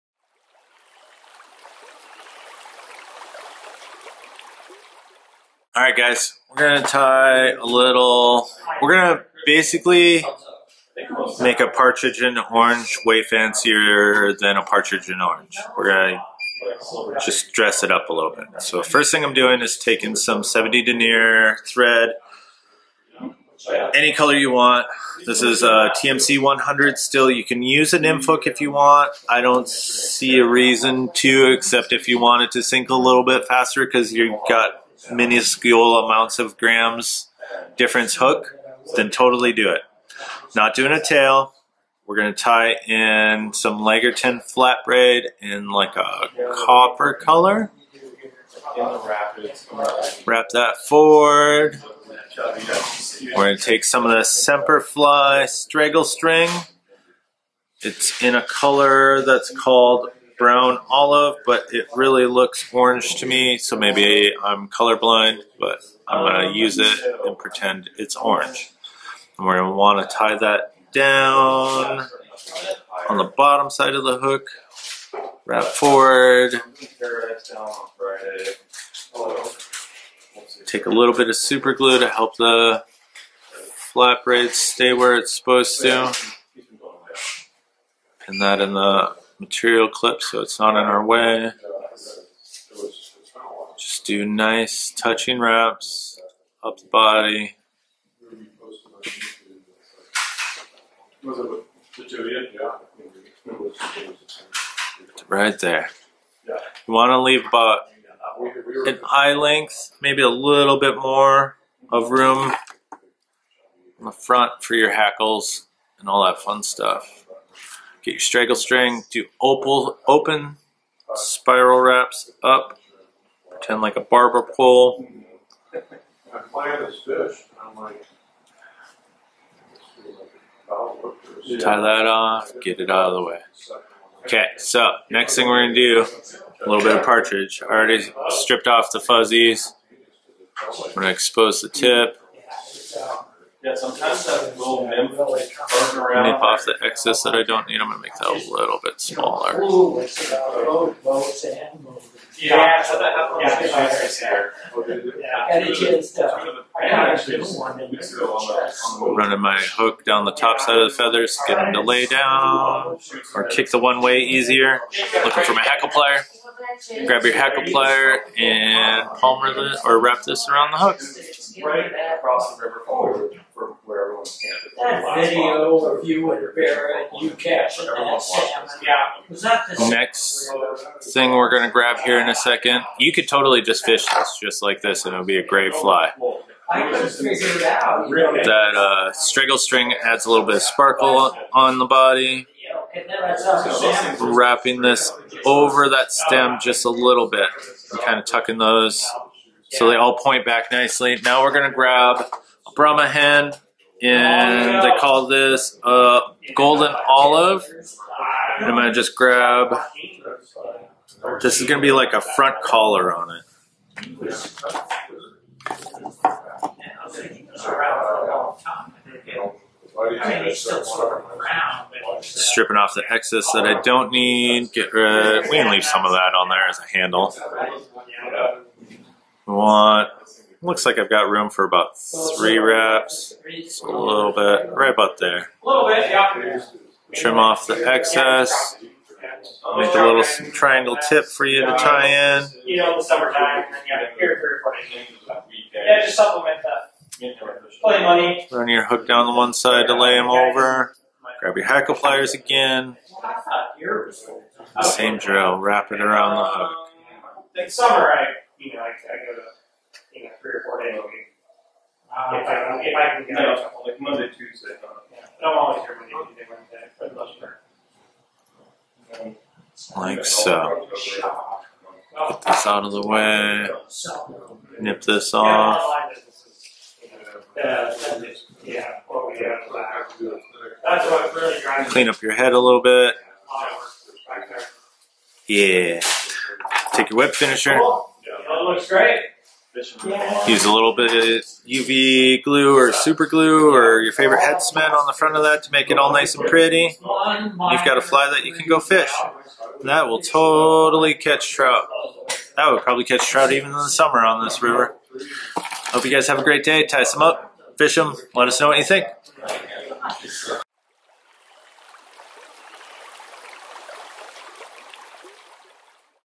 Modern Twist on a 100 Year Old Classic: Dark Cahill Style Orange Wet Fly
They say the only constant is change and that seems to hold true here in the shop. With this tie, we're adding a twist to a classic with some modern materials. The Cahill pattern stands the test of time, as it was initially tied by Dan Cahill over 100 years ago. The original Cahill dry pattern Dan tied imitated a Pale Morning or Evening Dun. This one imitates October Caddis and while there still may be a few kicking around into November, don't fear throwing these all fall and winter long.

Anyhow, we enjoy this simple beauty and think you might, too.

For more videos, reports, photos and event updates, visit us